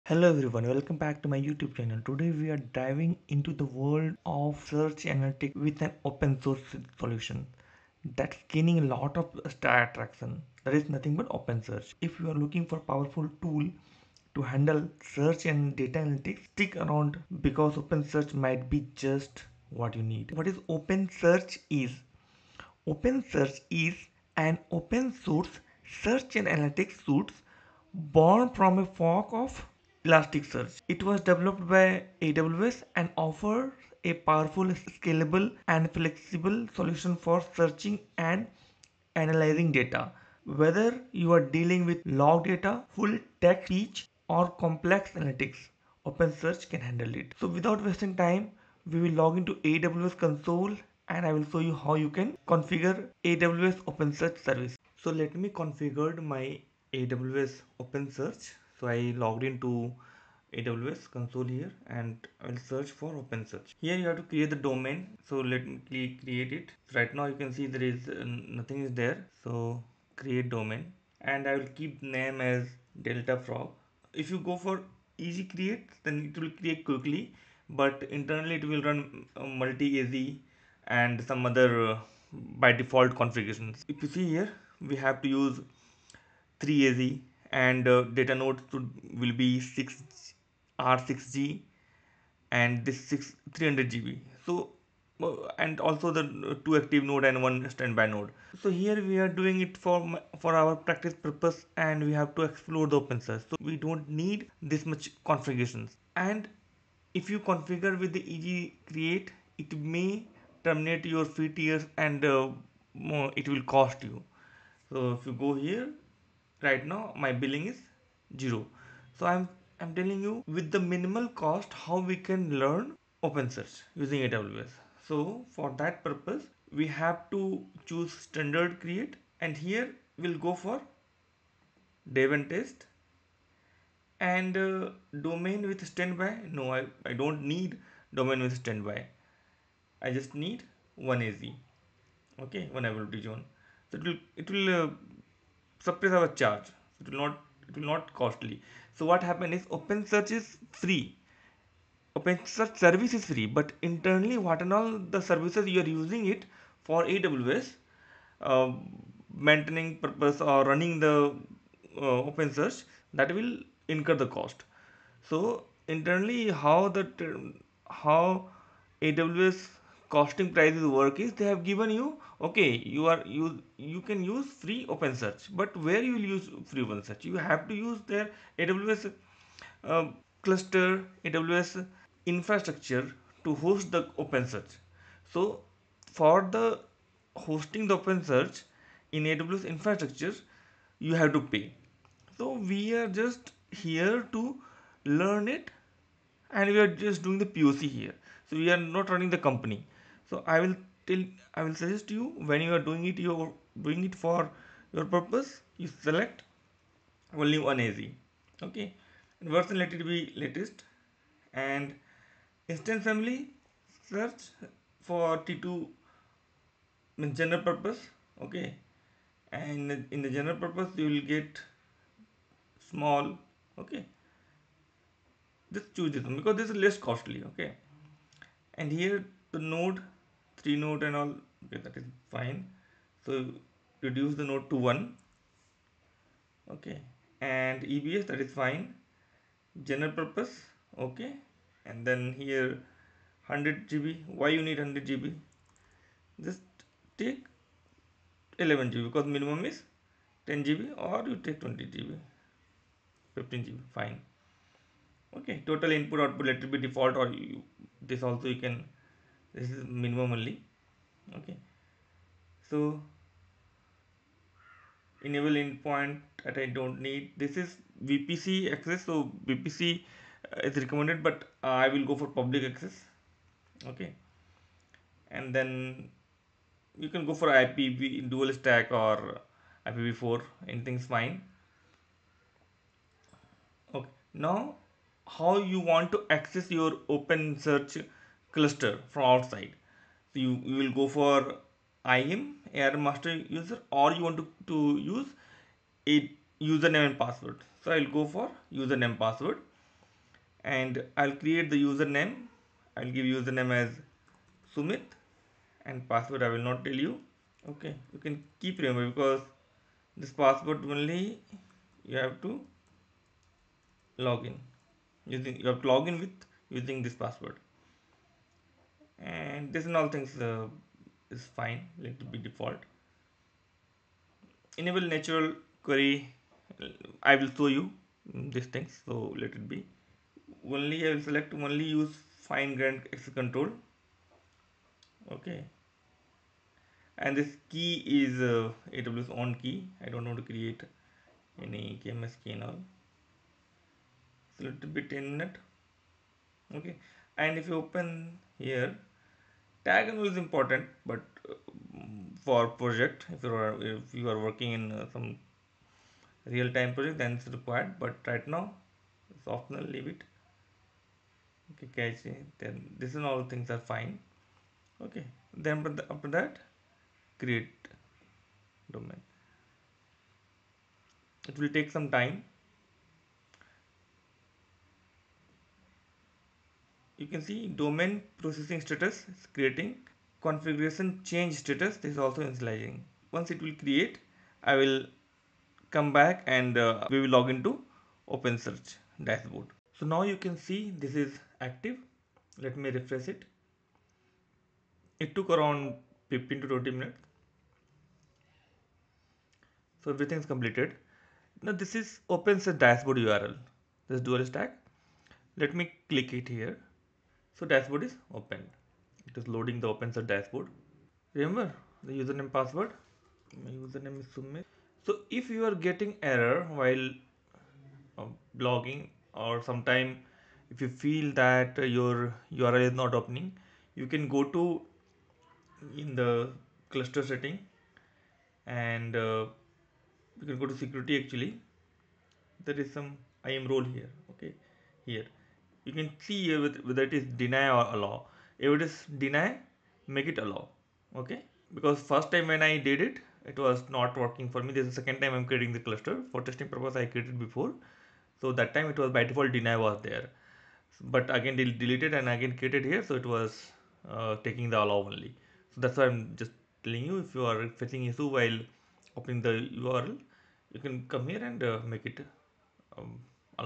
AWS OpenSearch Quick Start Guide from Scratch | Free Tier Tutorial
Welcome to our AWS OpenSearch Quick Start Guide! In this video, we'll walk you through setting up and using AWS OpenSearch from scratch, all on the Free Tier. Whether you're a beginner or looking to expand your cloud computing skills, this tutorial covers everything you need to get started with AWS OpenSearch.

We'll cover:

What is AWS OpenSearch?
Setting up an OpenSearch domain
Configuring access and security
Indexing and searching data
Tips for optimizing performance on the Free Tier
By the end of this guide, you'll have a solid understanding of AWS OpenSearch and be ready to start leveraging its powerful search capabilities for your projects.